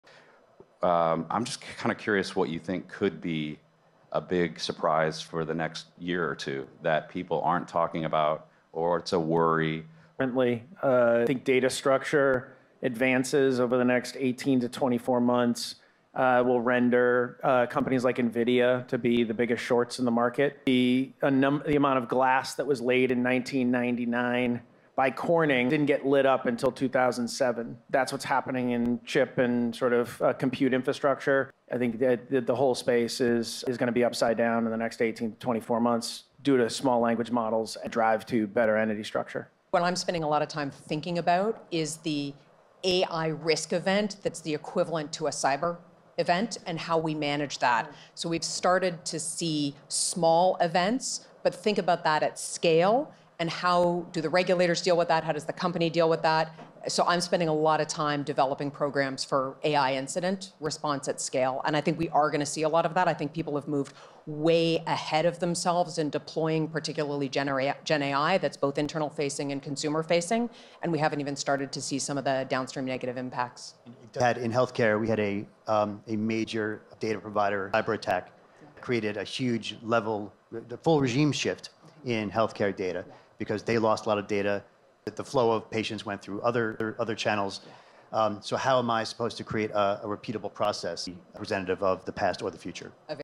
"Advances in data structure could disrupt the chip market, while AI incident risks loom ahead"
PANEL: The Future of KPI Creation - Data Integration and the Data Value Chain: Creating key performance indicators (KPIs) and integrating multiple data sources is critical for firms aiming to stay competitive. Innovative approaches to KPI creation, emphasizing the value chain and integration of diverse data sets and utilizing ML/AI. Best practices for leveraging data-driven insights to enhance alpha generation and operational efficiency.

Discover intricacies of KPI Creation - Data Integration and get the answers from our panel experts featuring:

Moderator
❖ Brian Engelbert CPA, CFA, Managing Director, Kyber Data Science, - TD Cowen
Speakers: ❖ Michael Fleder, Founder & CEO - Covariance.ai ❖ Sandy Balkin, Ph.D. , SVP, Strategy & Analytics - Royalty Pharma ❖ Kirk McKeown, Co-Founder - Carbon Arc ❖ Dera J. Nevin - Managing Director, FTI Consulting ❖ Sunaina Swaminathan, VP Partnerships - M Science

Transcript: I'm just kind of curious what you think could be a big surprise for the next year or two that people aren't talking about or it's a worry. I think data structure advances over the next 18 to 24 months will render companies like NVIDIA to be the biggest shorts in the market. The amount of glass that was laid in 1999 by Corning didn't get lit up until 2007. That's what's happening in chip and sort of compute infrastructure. I think that the whole space is going to be upside down in the next 18 to 24 months due to small language models and drive to better entity structure. What I'm spending a lot of time thinking about is the AI risk event that's the equivalent to a cyber event and how we manage that.

So, we've started to see small events, but think about that at scale and how do the regulators deal with that? How does the company deal with that? So, I'm spending a lot of time developing programs for AI incident response at scale. And I think we are going to see a lot of that. I think people have moved way ahead of themselves in deploying, particularly GenAI, that's both internal facing and consumer facing. And we haven't even started to see some of the downstream negative impacts. In healthcare, we had a major data provider cyber attack created a huge level, the full regime shift in healthcare data because they lost a lot of data. The flow of patients went through other channels. So how am I supposed to create a repeatable process representative of the past or the future?